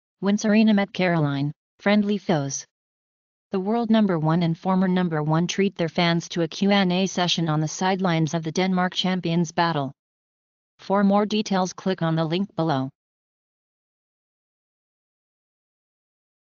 When Serena met Caroline: Friendly foes | By : CNN
The world number 1 and former number 1 treat their fans to a Q&A session on the sidelines of the Denmark Champions Battle.